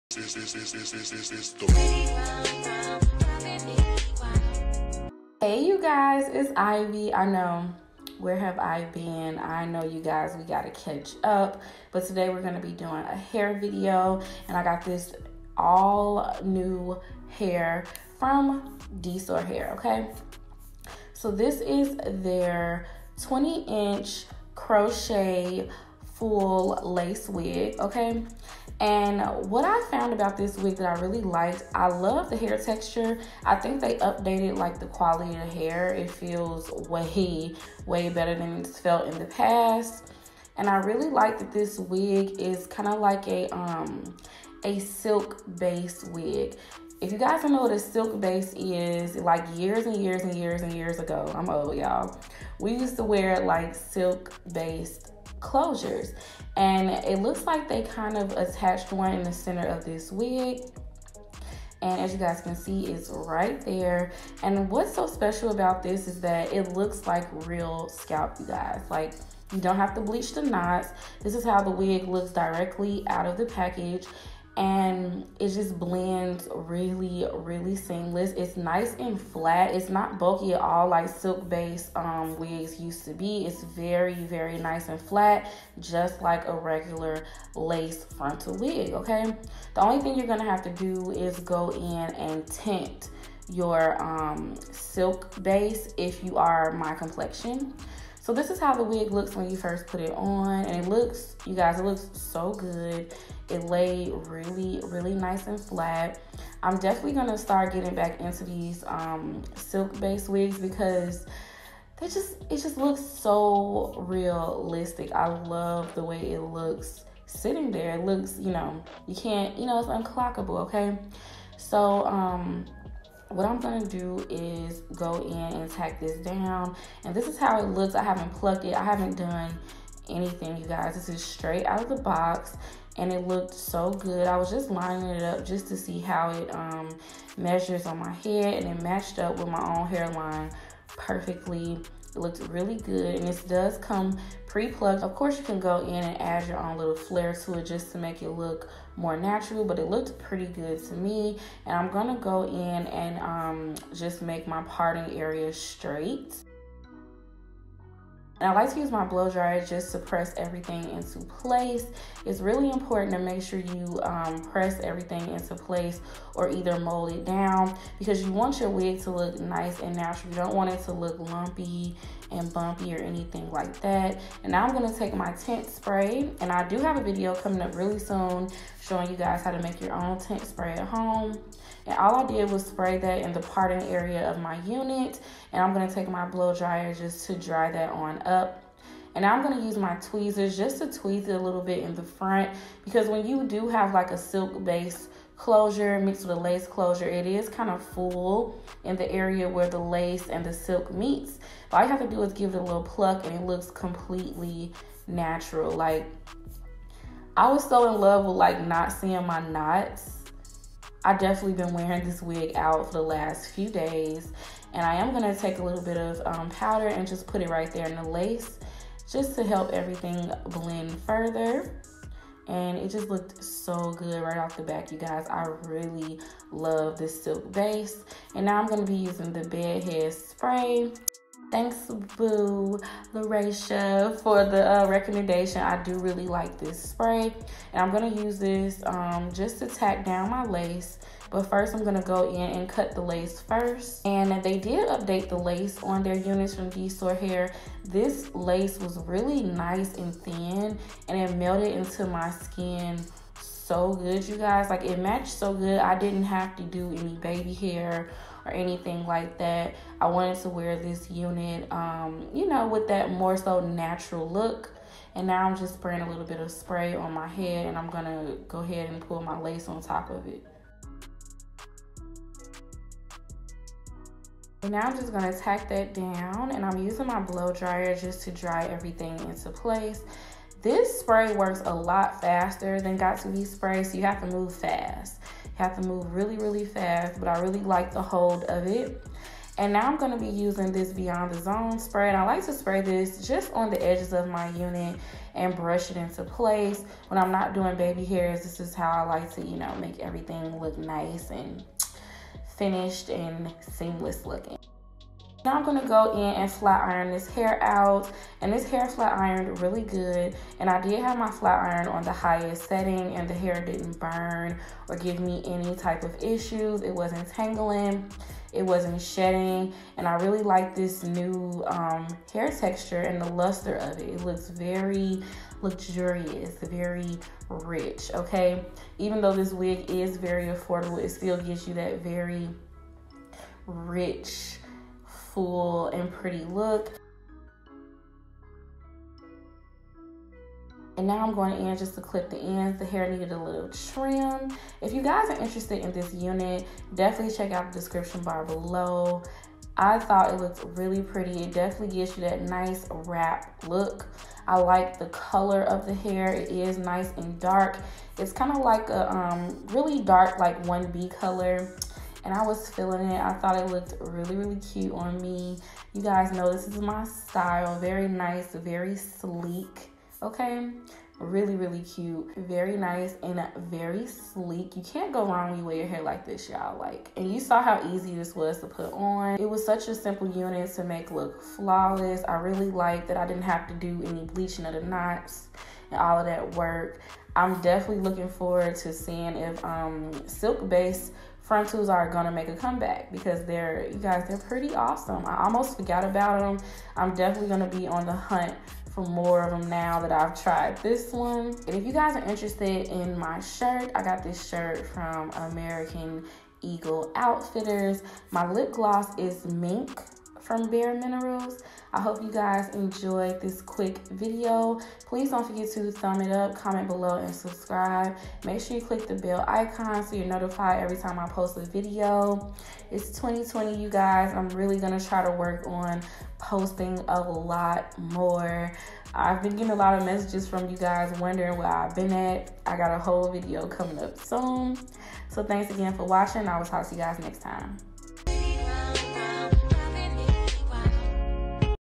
New Luxury Crochet Wig ! Are we Bringing Back silk closures Ft. DsoarHair
Hair Info: dsoar dsoar Luxuries Crocheting Straight Full Lace wig 20 inches

♥♥♥DSoar Hair Chinese New Year Sale ♥♥♥
➤OVER $189, GET Nightcap& Eyelash& Mini storage box